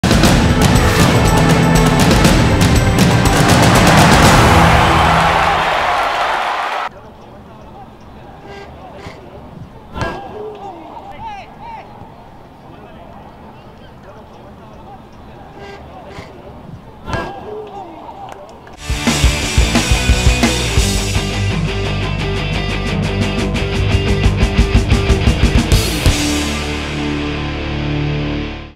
Así suena el poste cuando chuta Xavi
Impresionante chut de Xavi en el entrenamiento que acaba en el poste provocando un espectacular sonido